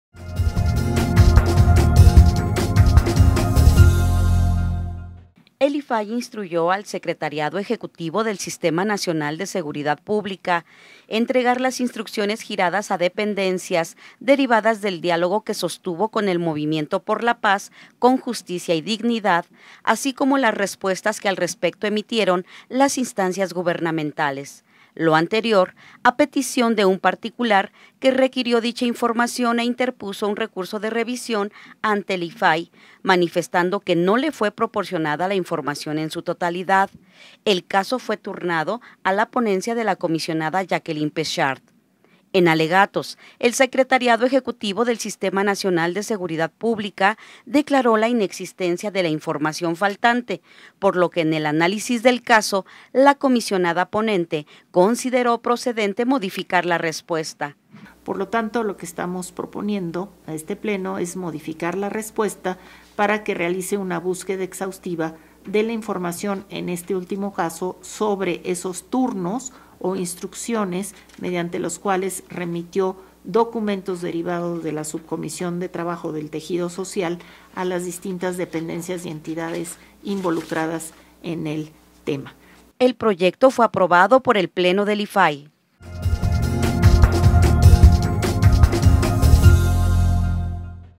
INSTRUYE IFAI AL SESNSP A ENTREGAR INFORMACIÓN
El Instituto Federal de Acceso a la Información y Protección de Datos (IFAI) instruyó al Secretariado Ejecutivo del Sistema Nacional de Seguridad Pública (SESNSP) buscar y, en su caso, entregar las instrucciones giradas a dependencias, derivadas del diálogo que sostuvo con el Movimiento por la Paz con Justicia y Dignidad, así como las respuestas que al respecto emitieron las instancias gubernamentales.